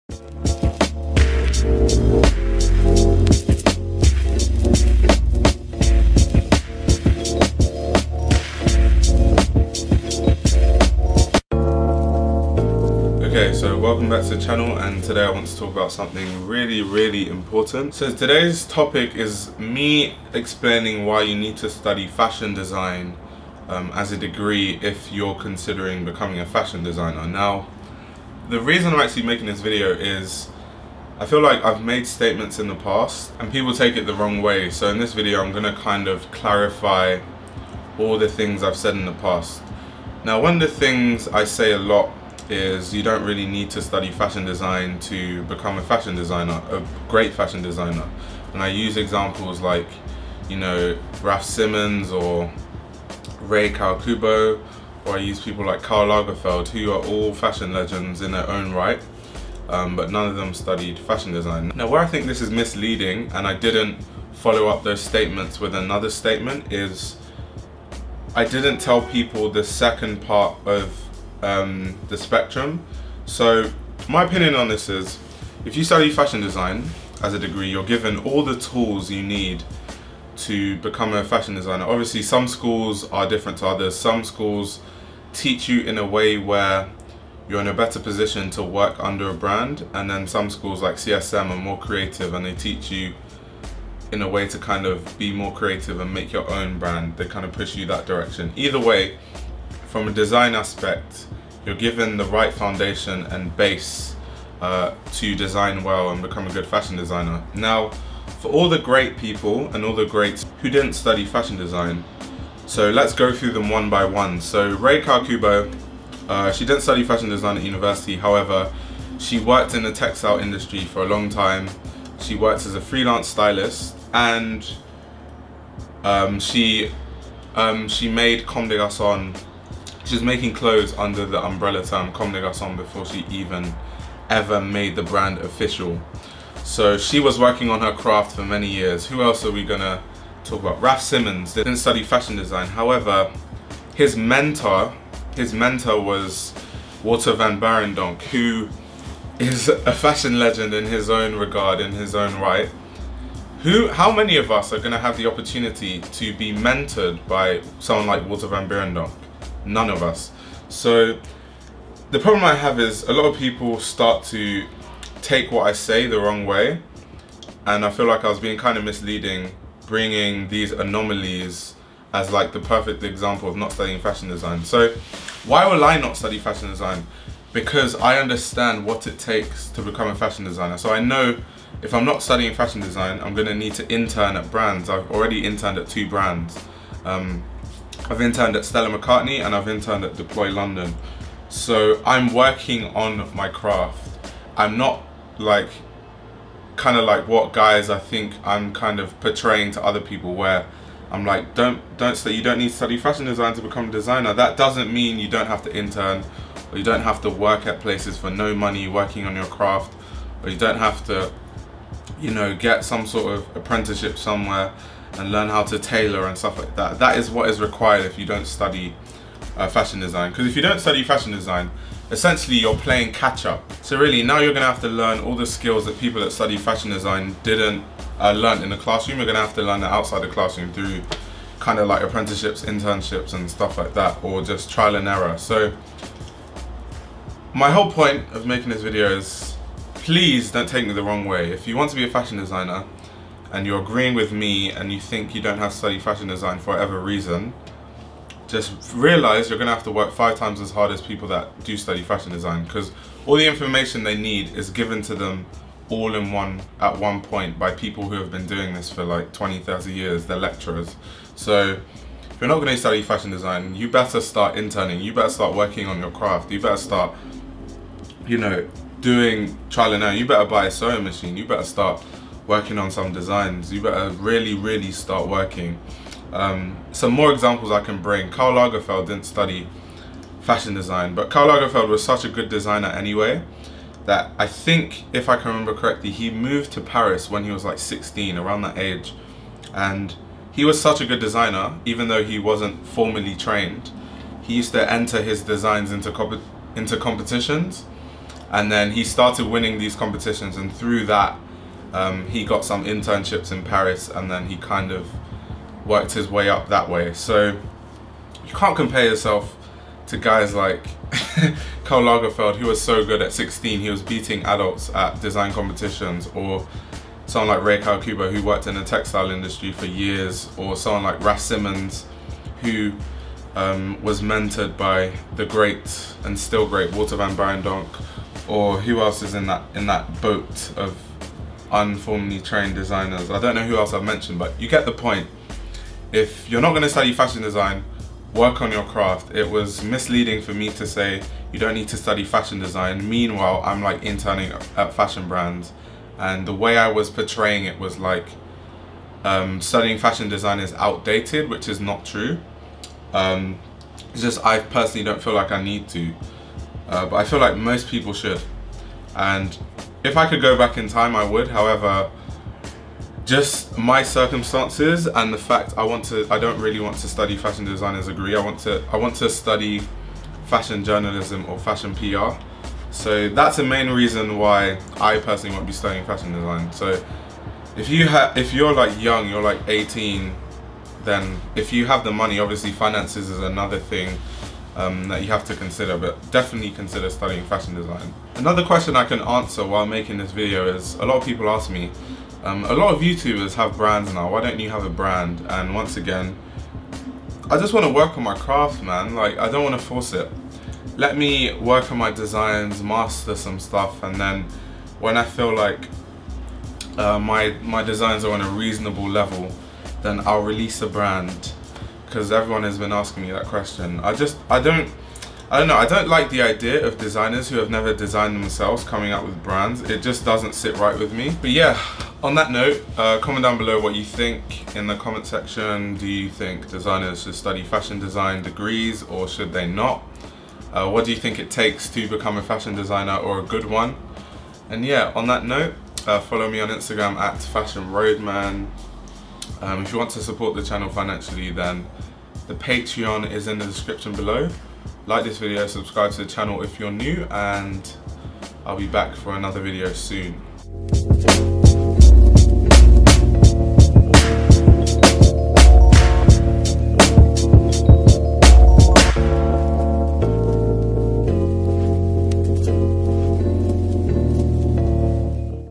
Why You NEED To Study Fashion Design At School
In today's video I am explaining why you should study fashion design. I feel like it is misleading using examples such as Karl Lagefeld, Rei Kawakubo & Raf Simons as reasons why it isn't necessary to study 'fashion design,' at university or college.